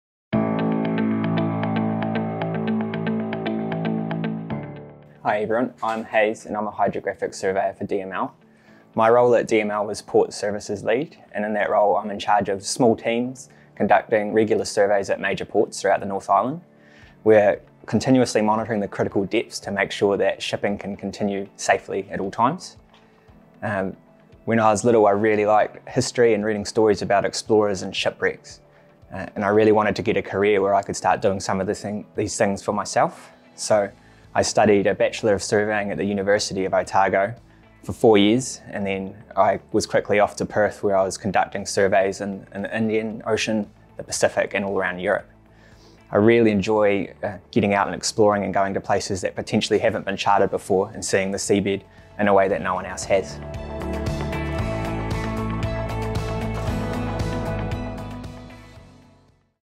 Meet Hayes
Hayes is a Senior Hydrographic Surveyor, Port Services Lead at Discovery Marine Limited.